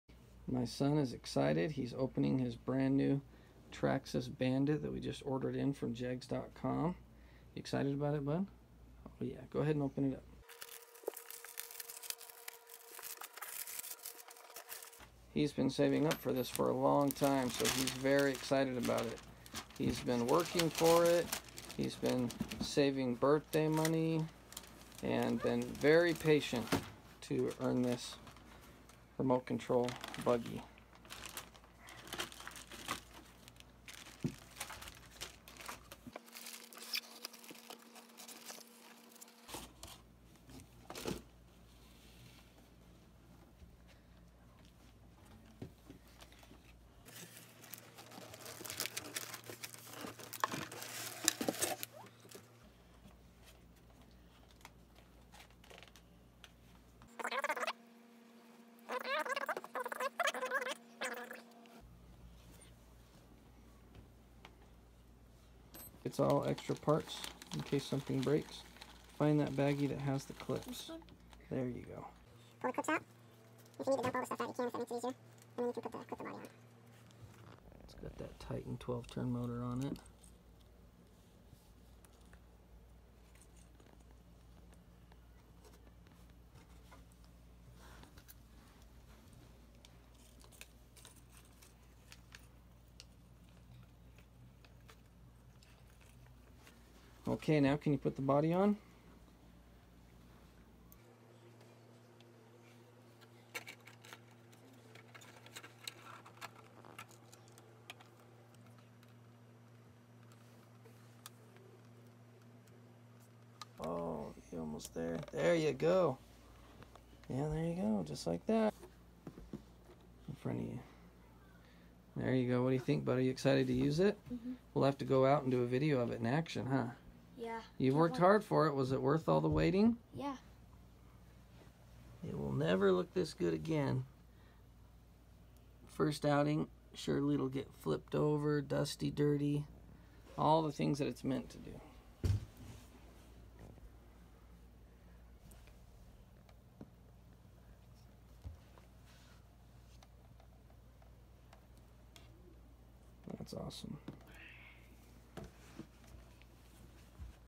UNBOXING - new TRAXXAS BANDIT
My son's new Traxxas Bandit arrived in the mail, and this video records the exciting moments that he unboxed his new RC. Thus far we are very impressed with Traxxas remote control cars and buggies.